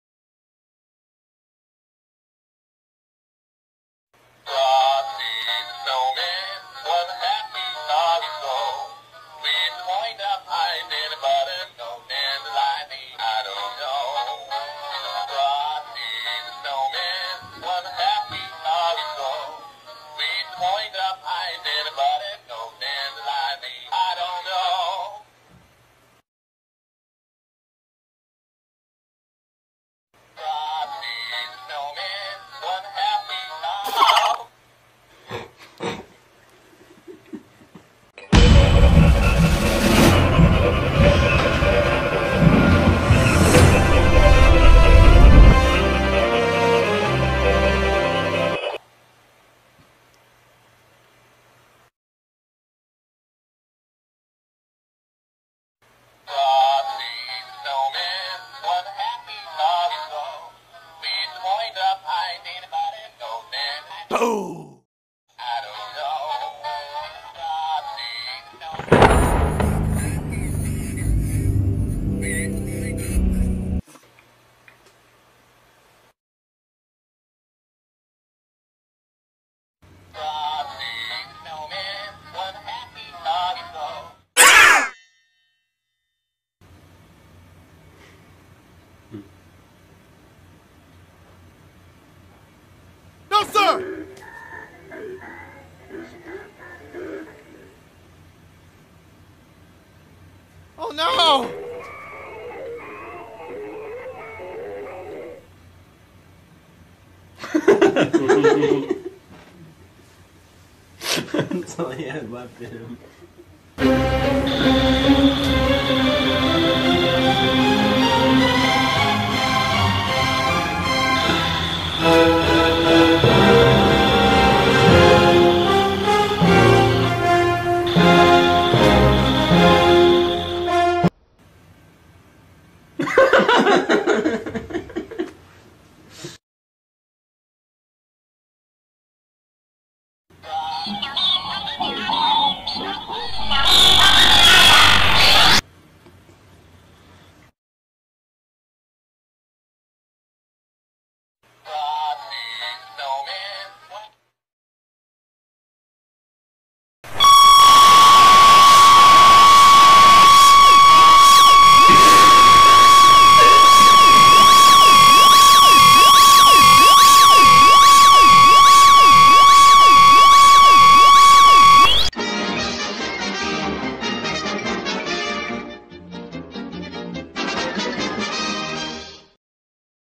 Frosty's Meltdown But Every Glitch Switches Even More Horrish Effects (Christmas In May Special)
Credit To businesswallaby And Others

This Is A Sequel Of Frosty's Meltdown But Every Glitch Switches Effects For Fun!

This Video Has Edited For Weeks Ago But I Never Published It So I Published It Anyway.